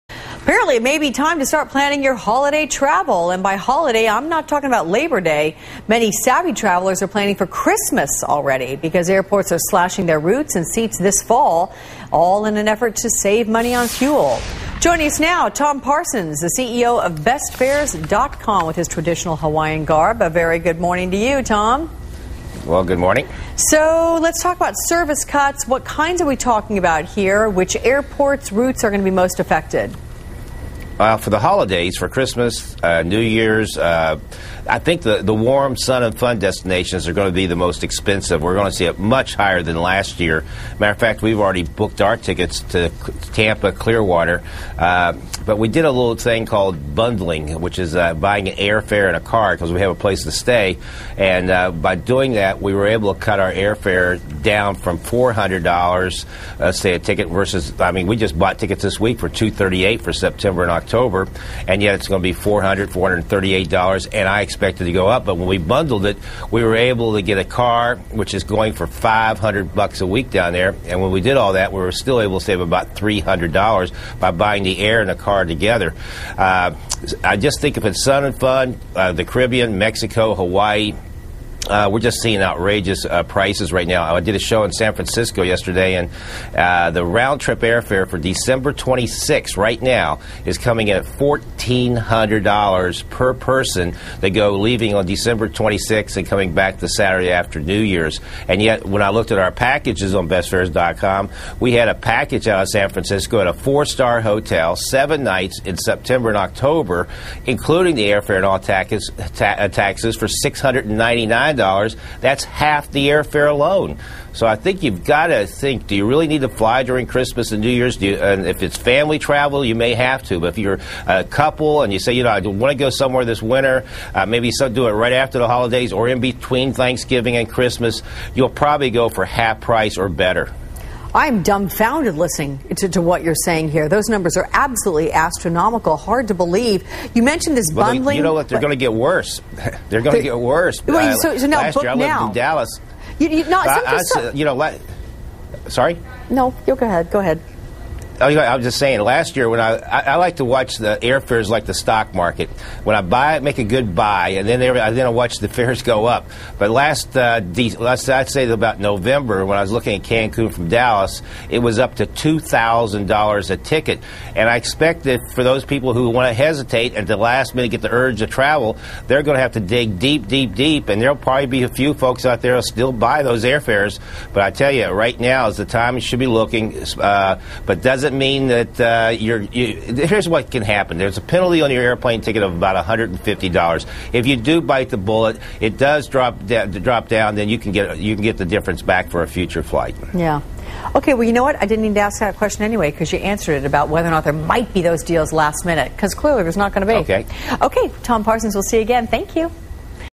MSN Video Holiday Travel Plans
Winter vacation
July 20: Tom Parsons with Bestfares.com talks about booking your winter vacation now.